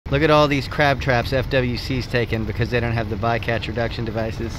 FWC TAKING A TON OF BLUE CRAB TRAPS IN FLORIDA
Saw FWC taking a ton of blue crab traps in Florida last week.